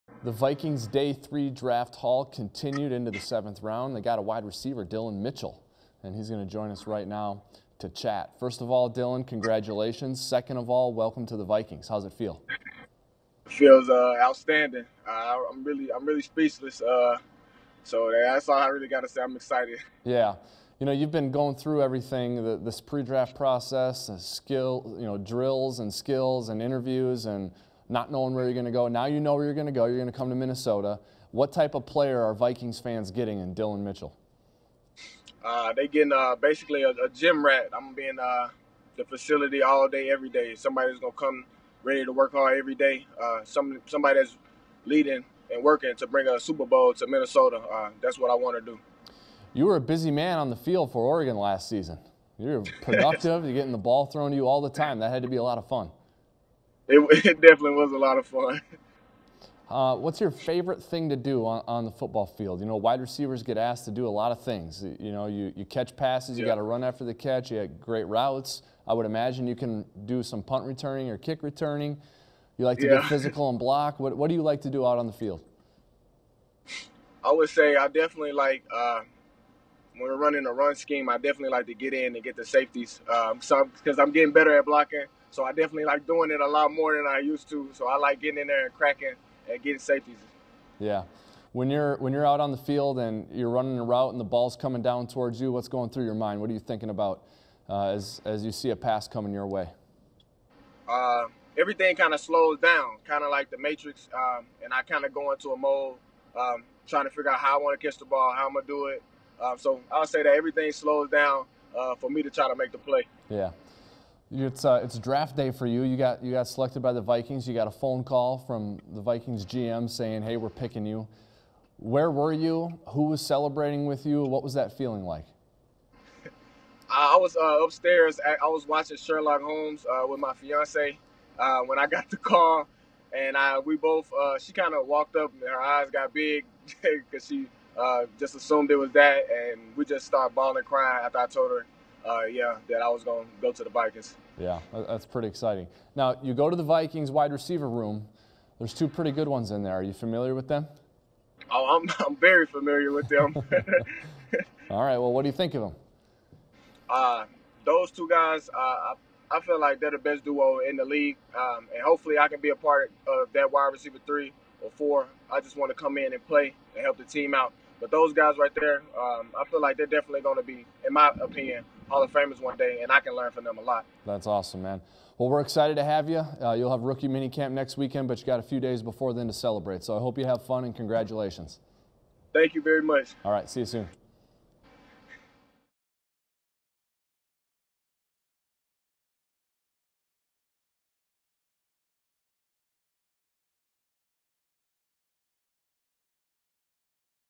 Dillon Mitchell: I'm Going To Work Hard Every Day To Bring A Super Bowl To Minnesota | Vikings
One of the newest Minnesota Vikings, Oregon WR Dillon Mitchell, FaceTimed in to TCO Studios on Saturday afternoon to chat with vikings.com's Mike Wobschall about what Vikings fans can expect from him, what the strengths are in his game, and more.